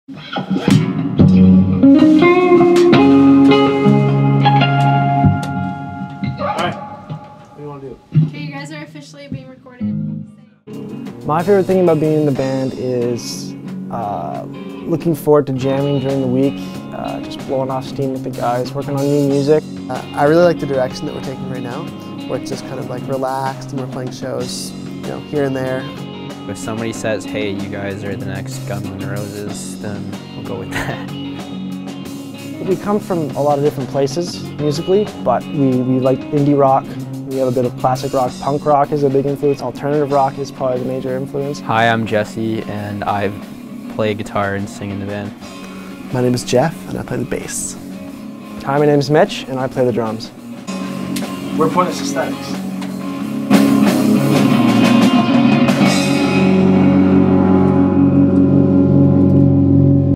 Pointless Aesthetics: The Band
A 60 second film project about the Carleton University born band Pointless Aesthetics.

Shot and edited by:
Yasmeen Kazak
Victoria Senetchko

Special thanks to:
Jeff Thorsland
Mitch Sawyer
Jesse Scheult

Connor Smith